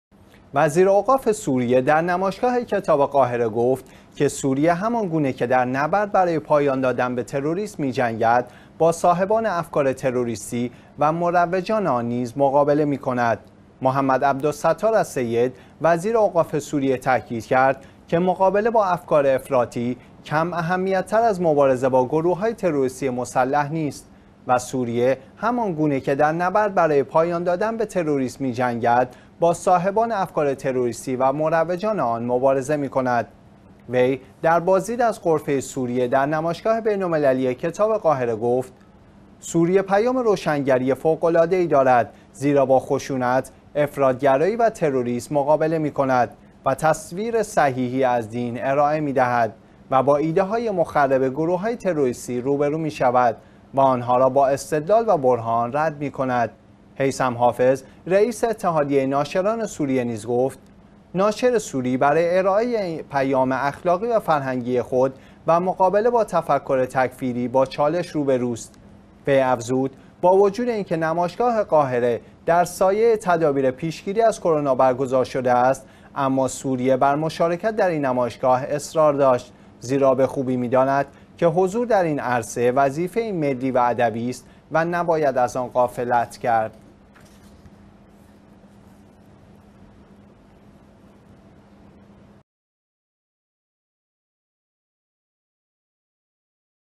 تأکید وزیر اوقاف سوریه بر مقابله با افکار افراطی
وزیر اوقاف سوریه در نمایشگاه کتاب قاهره گفت که سوریه همان گونه که در نبرد برای پایان دادن به تروریسم می‌جنگد، با صاحبان افکار تروریستی و مروجان آن نیز مقابله می‌کند.
"محمد عبدالستار السید" وزیر اوقاف سوریه تأکید کرد که مقابله با افکار افراطی کم اهمیت‌ تر از مبارزه با گروه‌های تروریستی مسلح نیست و سوریه همان گونه که در نبرد برای پایان دادن به تروریسم  می‌جنگد، با صاحبان افکار تروریستی و مروجان آن مبارزه می‌کند.
وی در بازدید از غرفه سوریه در نمایشگاه بین‌المللی کتاب قاهره، گفت: سوریه پیام روشنگری فوق‌العاده‌ای دارد، زیرا با خشونت، افراط‌گرایی و تروریسم مقابله می‌کند و تصویر صحیحی از دین ارائه می‌دهد و با ایده‌های مخرب گروه‌های تروریستی روبه‌رو می‌شود و آنها را با استدلال و برهان رد می‌کند.
هیثم حافظ ، رئیس اتحادیه ناشران سوریه نیز گفت: ناشر سوری برای ارائه پیام اخلاقی و فرهنگی خود و مقابله با تفکر تکفیری با یک استدلال با چالش‌ها روبه‌رو است.
وی افزود: با وجود اینکه نمایشگاه قاهره در سایه تدابیر پیشگیری از کرونا برگزار شده است، اما سوریه بر مشارکت در این نمایشگاه اصرار داشت، زیرا به خوبی می‌داند که حضور در این عرصه، وظیفه‌ای ملی و ادبی است و نباید از آن غفلت کرد.